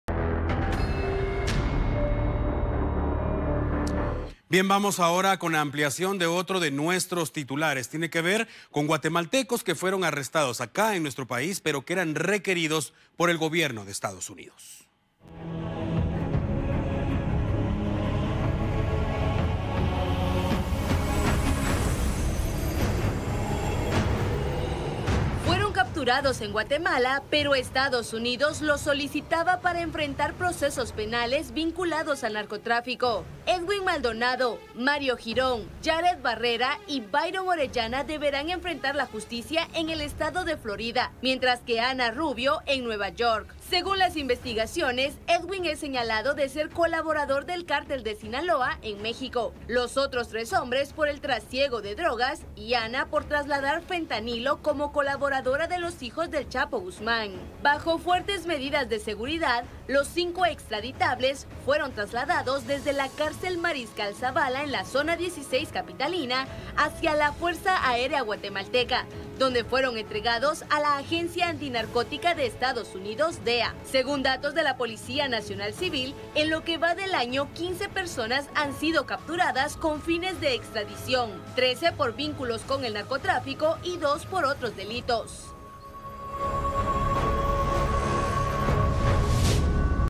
4 hombres y 1 mujer fueron trasladados a Estados Unidos
Fueron capturados en Guatemala, pero Estados Unidos los solicitaba para enfrentar procesos penales vinculados al narcotráfico. Bajo fuertes medidas de seguridad, los cinco extraditables fueron trasladados desde la cárcel Mariscal Zavala en la zona 16 capitalina hacia la Fuerza Aérea Guatemalteca, donde fueron entregados a la DEA.